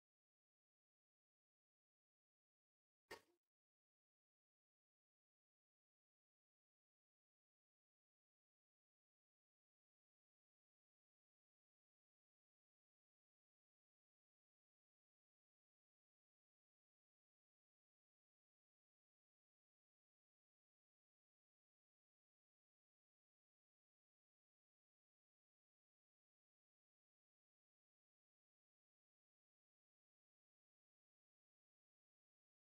Wyrmwick Splinted Greaves
This video shows our Polyurethane (PU) Splinted Greaves.  These are shown in our Steel finish.  They can be purchased on our